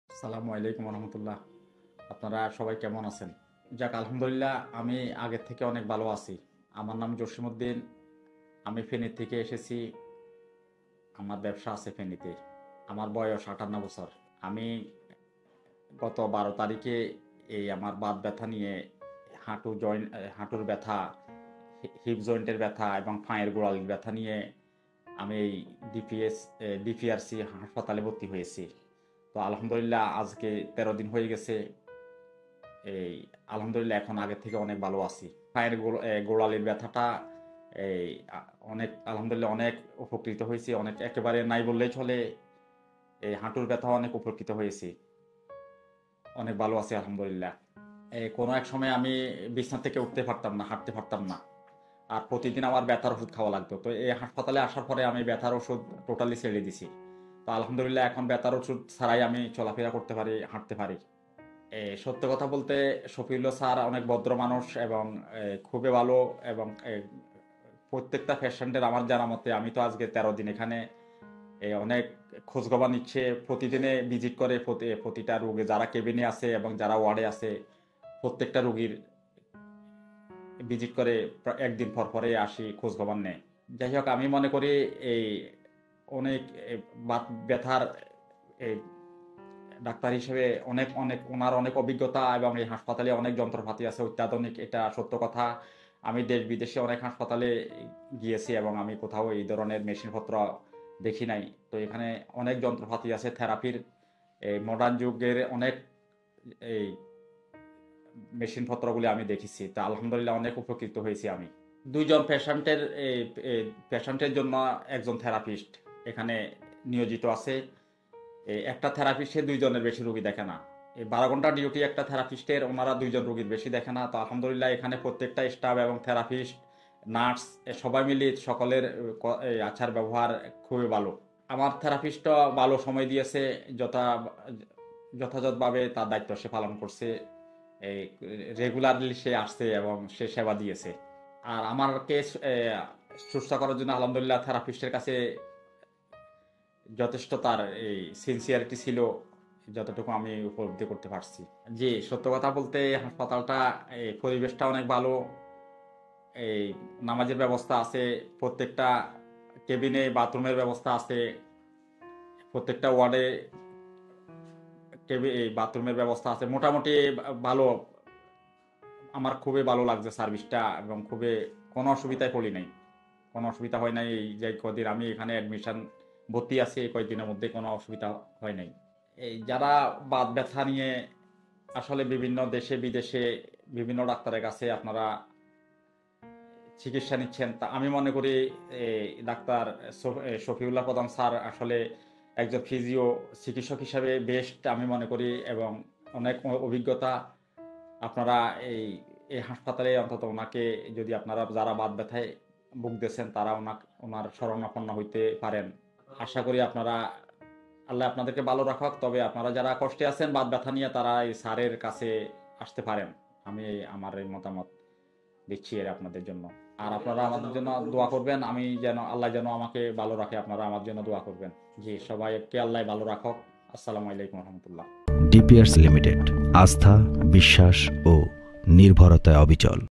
হিপ জয়েন্ট, পায়ের গোড়ালী ও হাটু ব্যথার রোগী সুস্থ হয়ে বাড়ি ফিরে যাচ্ছে | Dr.Md.Shafiullah Prodhan
✺আমাদের  এর ঠিকানাঃ-
DPRC
12/1 Ring Road, shyamoli Dhaka-1207, Bangladesh.
(ডিপিআরসি , 12/1  রিং রোড শ্যামলি ঢাকা-১২০৭ বাংলাদেশ)